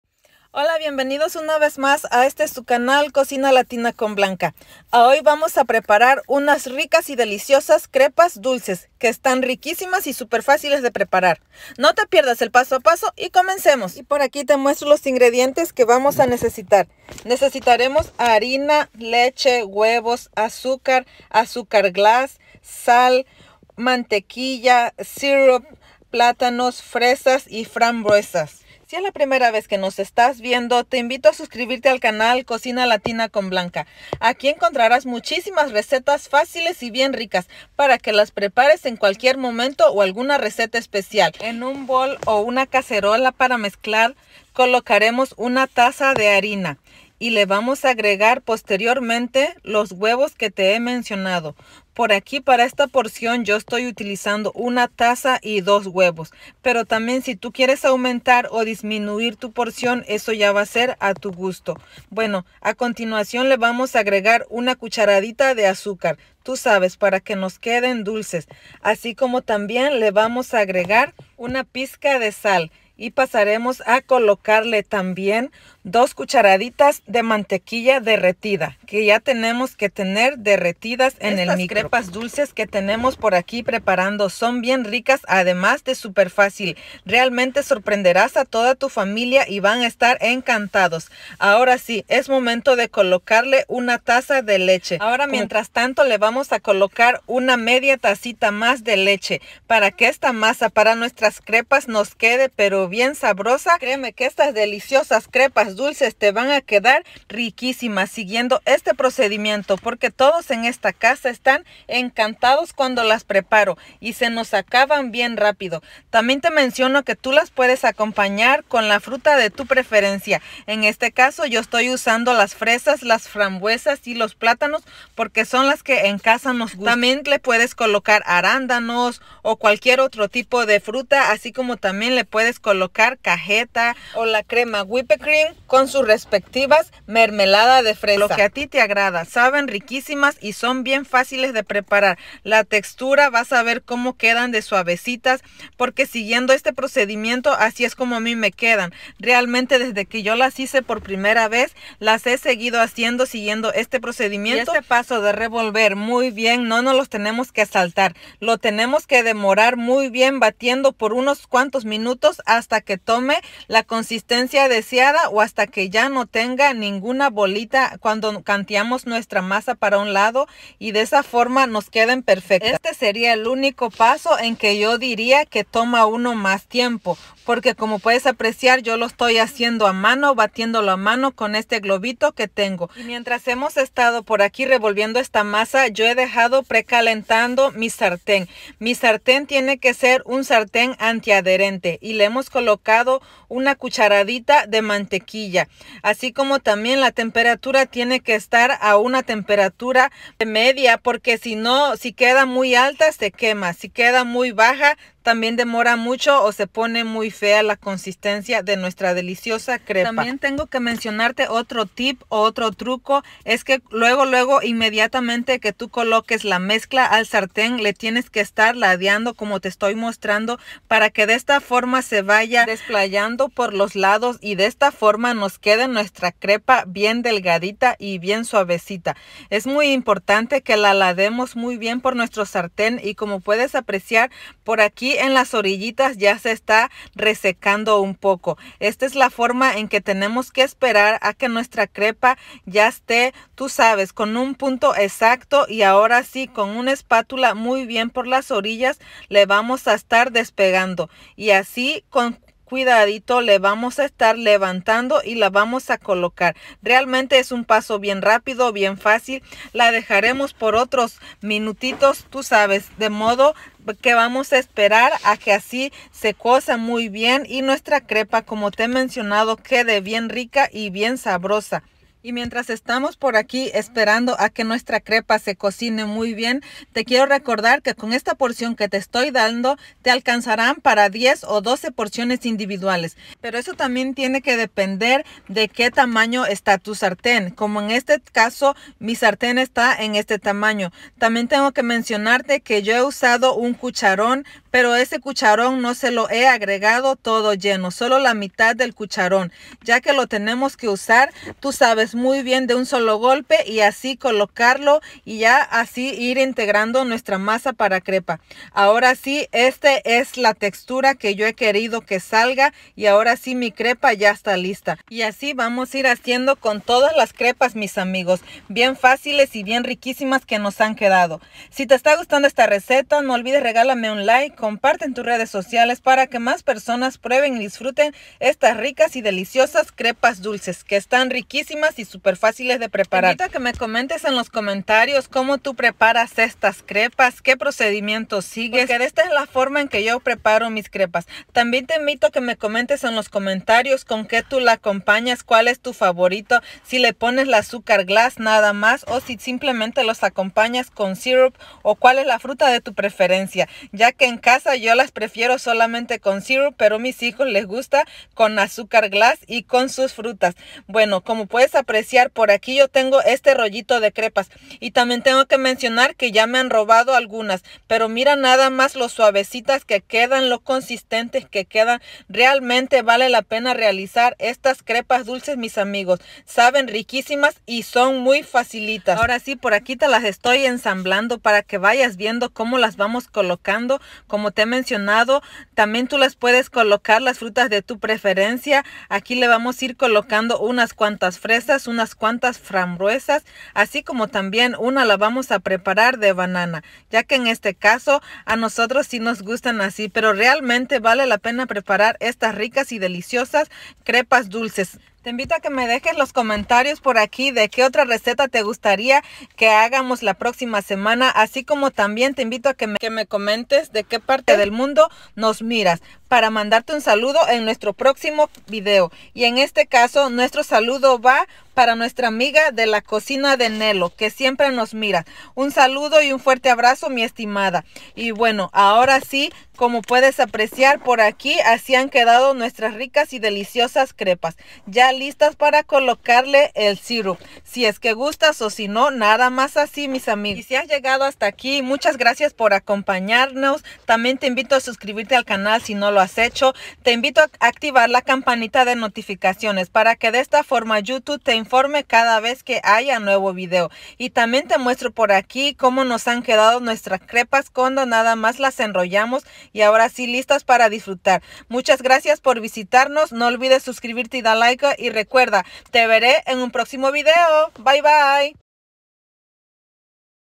CREPAS DULCES || Masa Para Crepas Riquísimas y Tiernas
Crepas Dulces. Crepas Dulces o CREPES DULCES Súper Riquísimos y Bien Fáciles de preparar. Sorprenderas a todos en tu casa con esta delicia de Crepes Dulces.

INGREDIENTES
1 taza de Harina todo uso
2 huevos
2 cucharadas de Mantequilla deretida
Azúcar 1 cucharada
Azúcar glass (al gusto)
Syrup (al gusto)
 1 pizca de Sal
Plátano (al gusto)
Frambuesas (al gusto )
Fresas (al gusto)